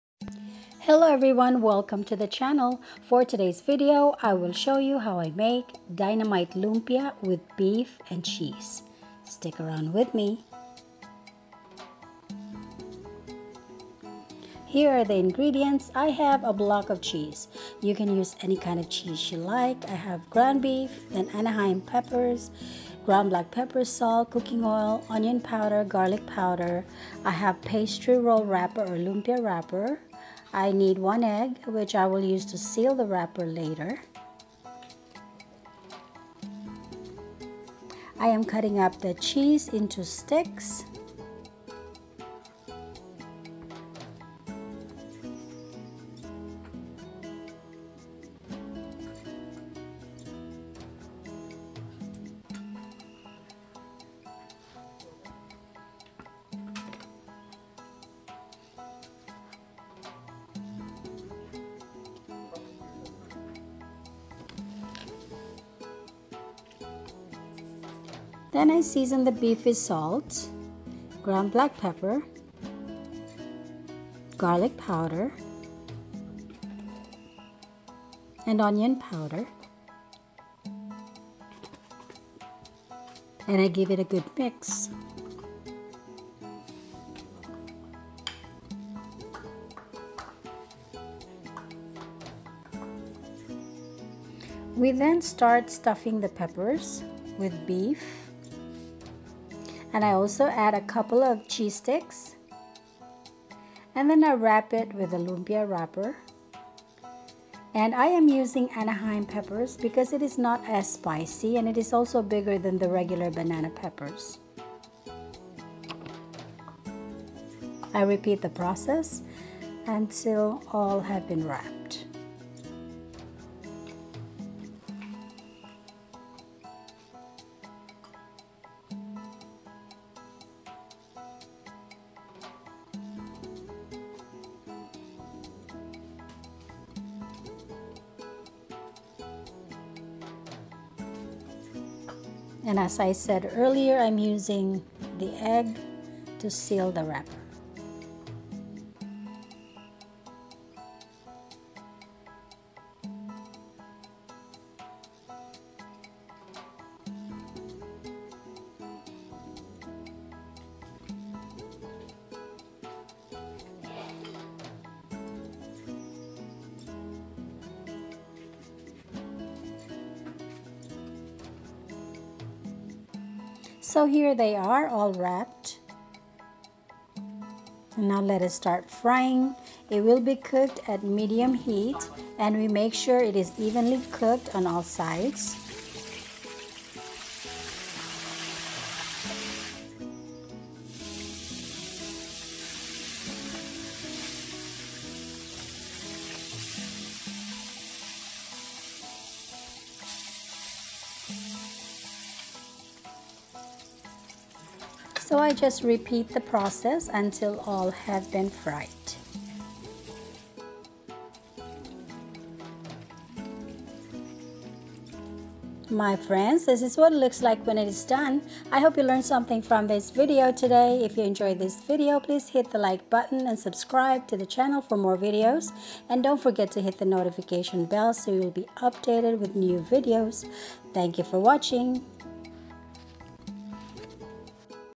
HOW TO COOK DYNAMITE LUMPIA WITH BEEF AND CHEESE
This dish is called dynamite because they are in rolls which look like dynamite and because it uses green peppers which can vary in kind, depending on whether you want it to be spicy hot or not. Some just put cheese without meat and some use pork instead of beef. They also vary on the kind of cheese used.